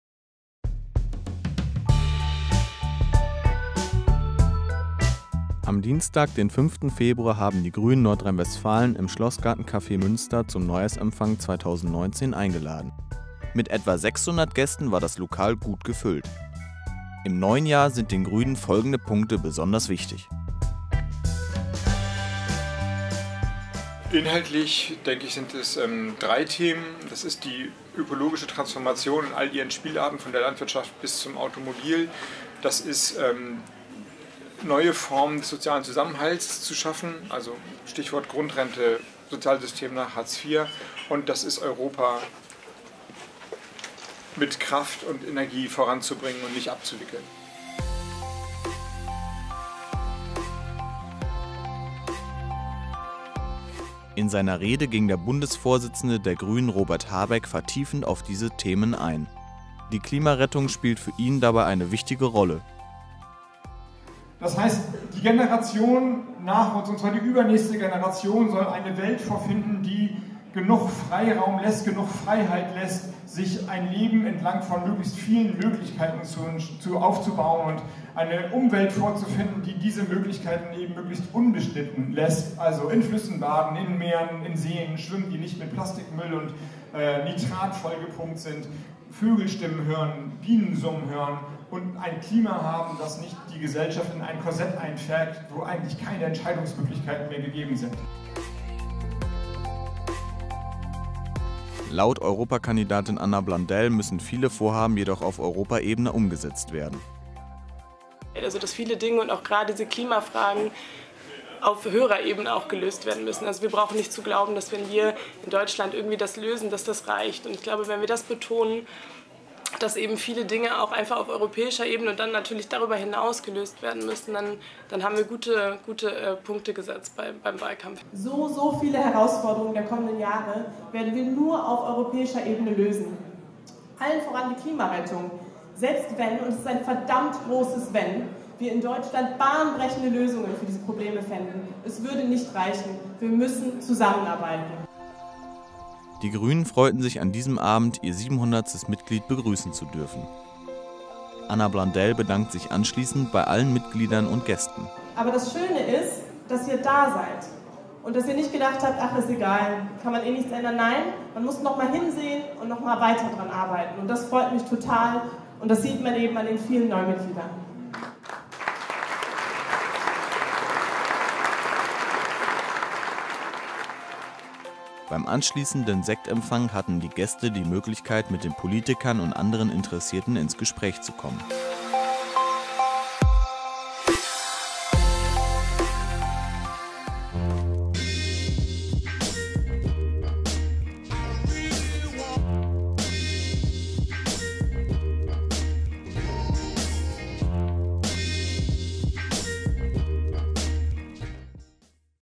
Neujahrsempfang der Grünen NRW | Ostviertel
Am 05. Februar fand im Schlossgarten Café der Neujahrsempfang statt. Neben Anna Blundell und Stephan Orth hielt unter anderem auch Robert Habeck eine Rede über die Punkte, die der grünen Partei in diesem Jahr besonders wichtig sind.

Ein Beitrag von Finn Reinhardt